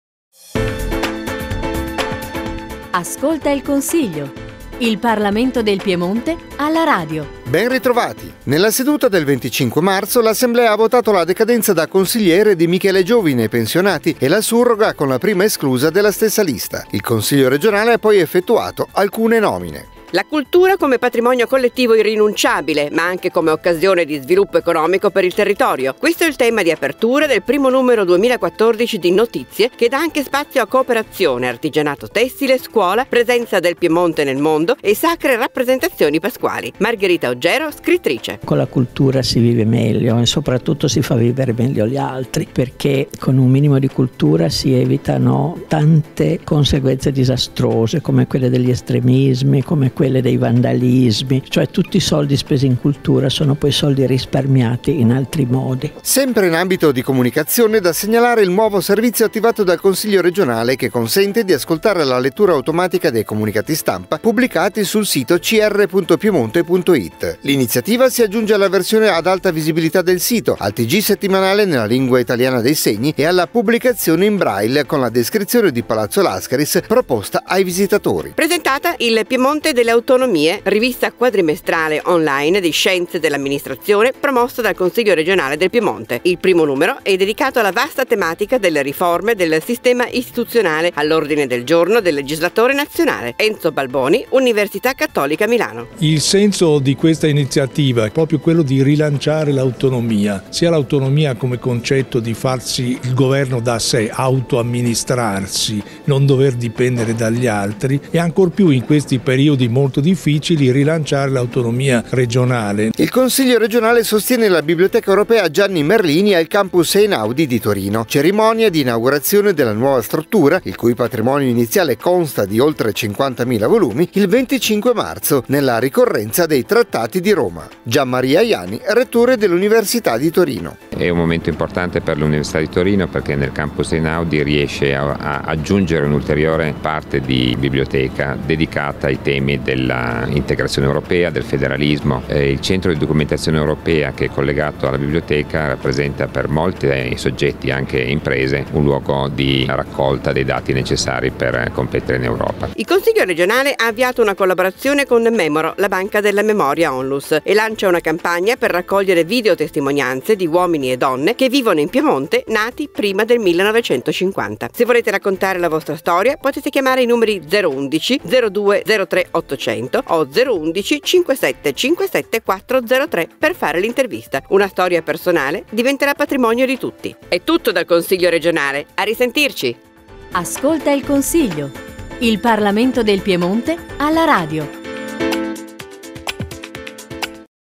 CRP Ascolta il Consiglio n.10 del 27 marzo 2014
Nella seduta del 25 marzo l'Assemblea ha votato la decadenza da consigliere di Michele Giovine (Pensionati) e la surroga con la prima esclusa della stessa lista. Il Consiglio regionale ha poi effettuato alcune nomine.

La cultura come patrimonio collettivo irrinunciabile, ma anche come occasione di sviluppo economico per il territorio: questo il tema di apertura del primo numero 2014 di Notizie che dà anche spazio a cooperazione, artigianato tessile, scuola, presenza del Piemonte nel mondo e sacre rappresentazioni pasquali.
Margherita Oggero scrittrice

Sempre in ambito comunicazione da segnalare il nuovo servizio attivato dal Consiglio regionale che consente di ascoltare la lettura automatica dei comunicati stampa pubblicati sul sito cr.piemonte.it.
L'iniziativa si aggiunge alla versione ad alta visibilità del sito, al tg settimanale nella Lingua italiana dei segni e alla pubblicazione in braille con la descrizione di Palazzo Lascaris, proposta ai visitatori.

Presentata "Il Piemonte delle Autonomie", rivista quadrimestrale "on line" di scienze dell'Amministrazione promossa dal Consiglio regionale del Piemonte. Il primo numero è dedicato alla vasta tematica delle riforme del sistema istituzionale, all'ordine del giorno del legislatore nazionale.
Enzo Balboni - Università Cattolica Milano

Il Consiglio regionale sostiene la Biblioteca Europea Gianni Merlini, al Campus Einaudi di Torino. Cerimonia di inaugurazione della nuova struttura, il cui patrimonio iniziale consta di oltre 50mila volumi, il 25 marzo, nella ricorrenza dei Trattati di Roma.
Gian Maria Ajani rettore Università di Torino

Il Consiglio regionale ha avviato una collaborazione con Mémoro La Banca della Memoria onlus e lancia una campagna per raccogliere video testimonianze di uomini e donne che vivono in Piemonte, nati prima del 1950.
Se volete raccontare la vostra storia potete chiamare i numeri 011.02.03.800 o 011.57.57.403 per fare l'intervista. Una storia personale diventerà patrimonio di tutti.